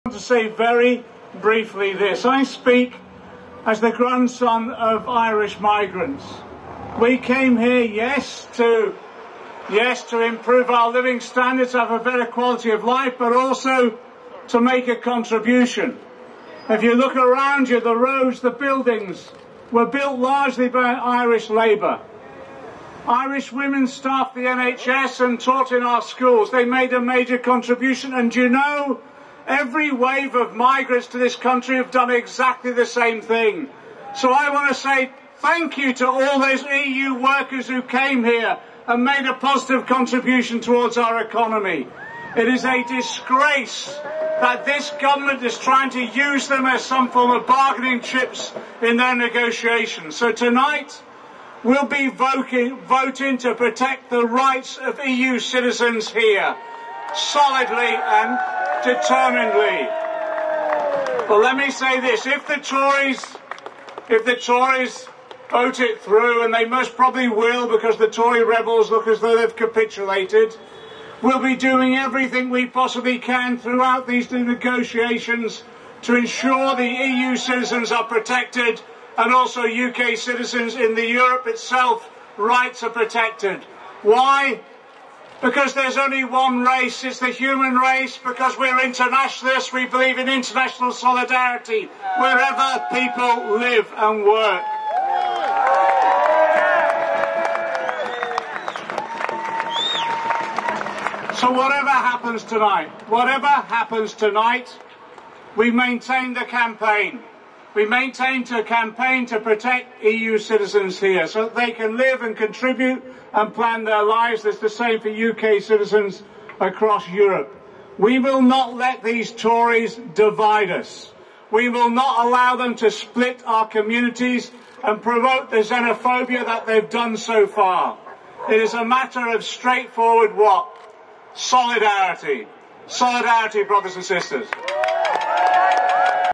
John McDonnell at Right to Remain rally.
John McDonnell at Right to Remain rally, Parliament Square, London 13th March 2017.